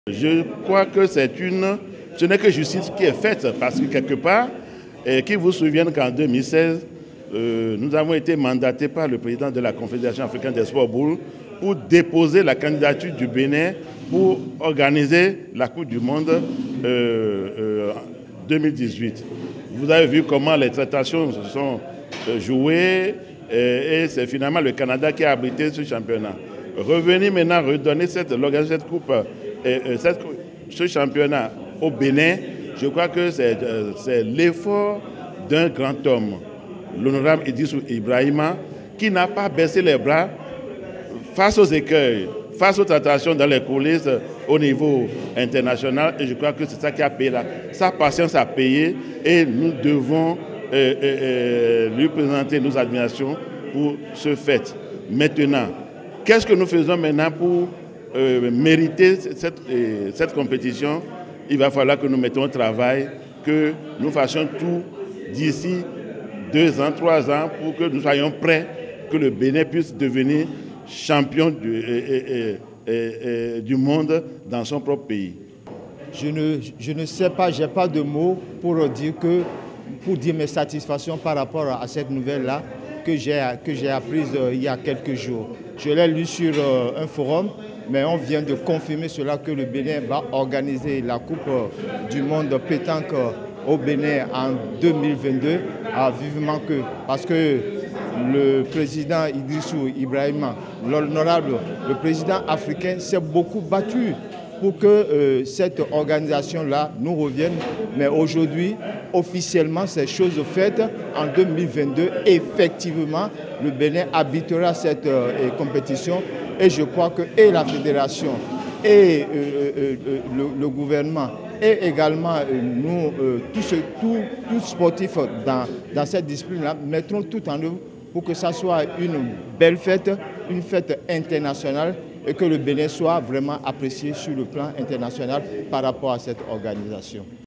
Le Bénin accueil le mondial 2022 de pétanque
Les Réactions  des présidents Vinou et Bide